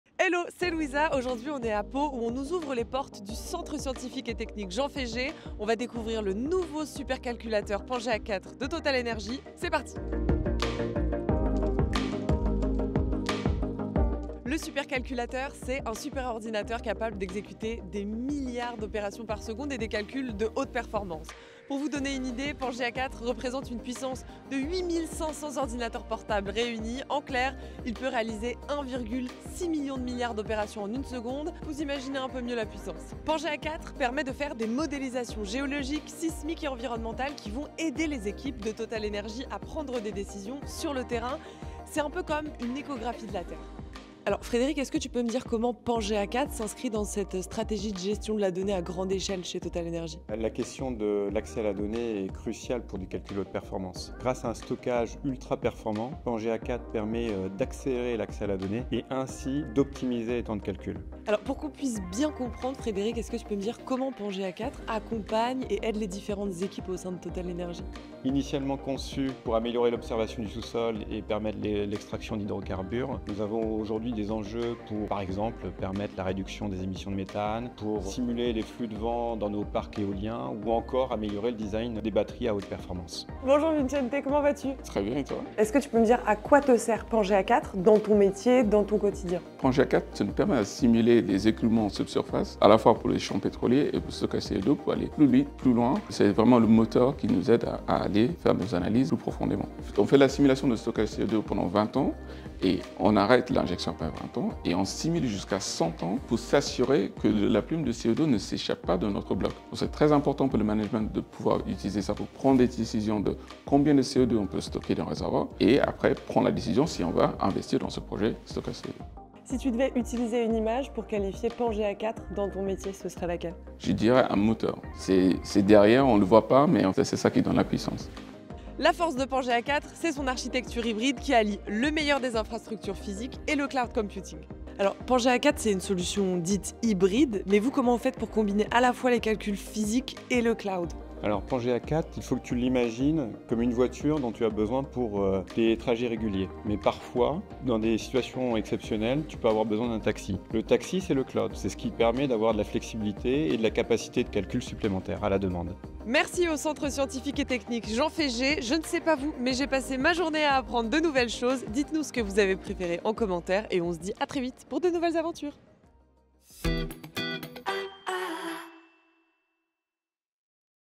Découvrez Pangea 4 - TotalEnergies
Découvrez Pangea 4 au Centre Scientifique et Technique Jean Féger à Pau. Avec une puissance théorique de 31,7 pétaflops, notre supercalculateur hybride révolutionne la modélisation géologique et environnementale.